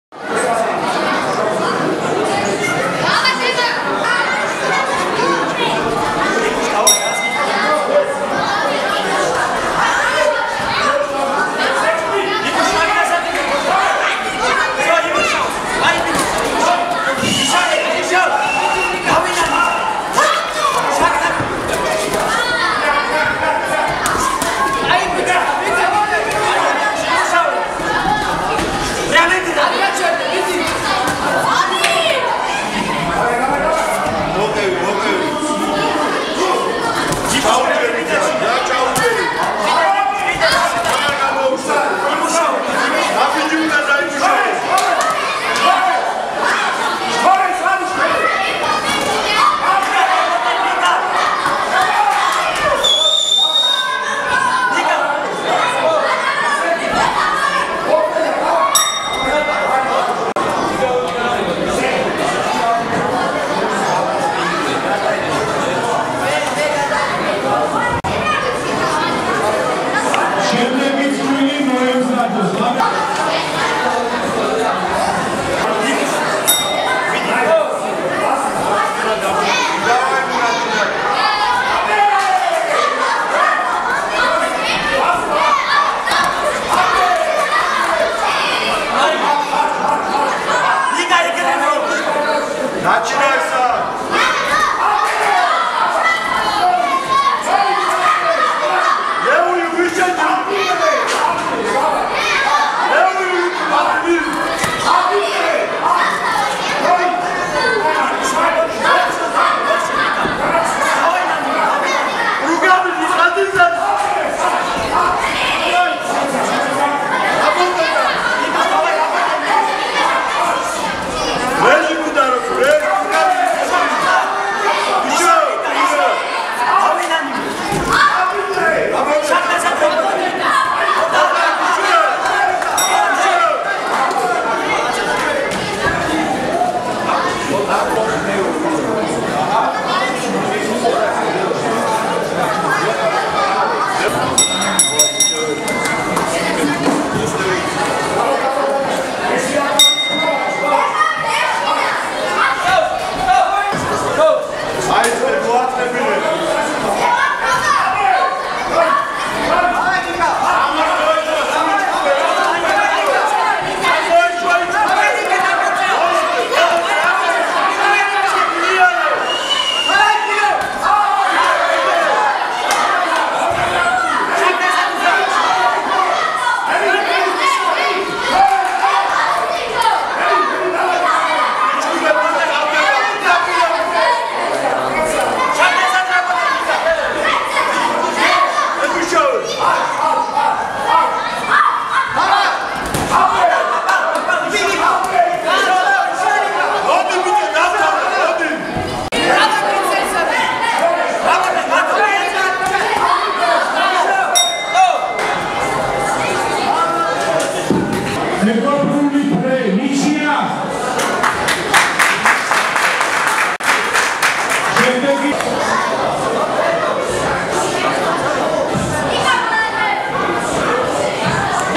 Բռնցքամարտի միջազգային կարգի առաջնություն 38կգ Գեղամ Ահարոնյան (Մարտունի)-Նիկա Ասլանյան (Թբիլիսի)
Սպարտանցի ակումբի բռնցքամարտի միջազգային կարգի առաջնություն 38կգ Գեղամ Ահարոնյան (Մարտունի-Ն. Գետաշեն)-Նիկա Ասլանյան (Թբիլիսի Սպարտանցի) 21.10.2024թ. Թբիլիսի